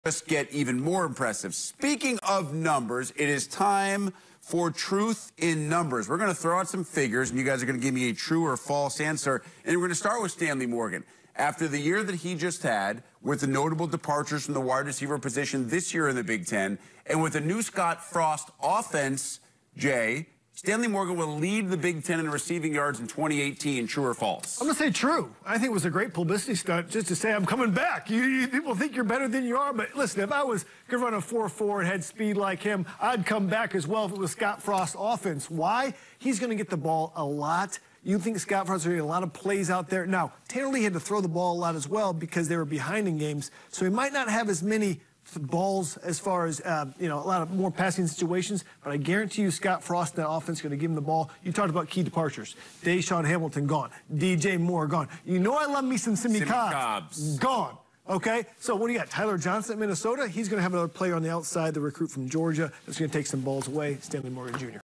Will Stanley Morgan Jr. Lead B1G in Receiving Yards in 2018?
J Leman discusses whether Nebraska's Stanley Morgan Jr. will lead B1G in receiving yards in 2018.